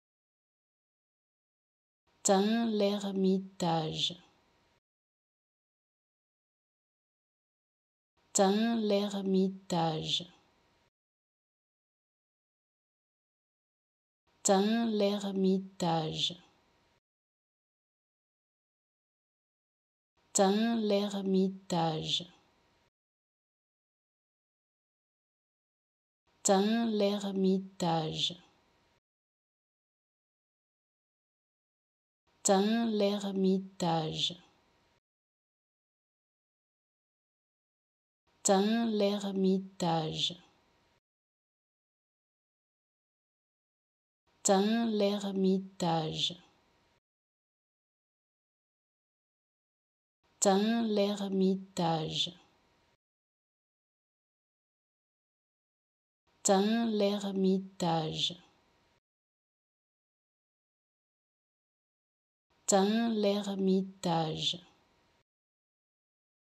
How to Pronounce ''Tain-l'Hermitage'' Correctly in French
Learn how to say and properly pronounce ''Tain-l'Hermitage'' in French with this free pronunciation tutorial. Apprenez à prononcer des mots en français grâce à nos tutoriels.